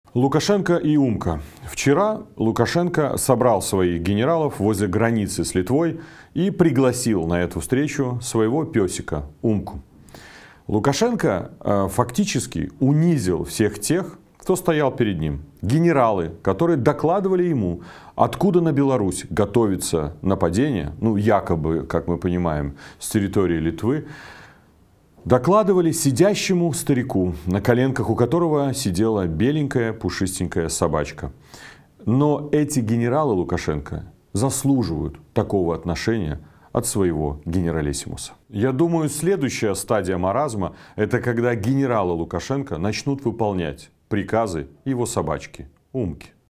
Лукашенко с собачкой
Диктатор собрал своих генералов возле границы с Литвой и пригласил на эту встречу своего песика Умку.

Лукашенко, фактически, унизил всех тех, кто стоял перед ним. Генералы докладывали о том, "откуда на Беларусь готовится нападение" сидящему старику, на коленках у которого сидела беленькая, пушистенькая собачка. Но эти генералы Лукашенко заслуживают такого отношения от своего генералиссимуса.

Думаю, что следующая стадия маразма — это когда генералы Лукашенко начнут выполнять приказы его собачки.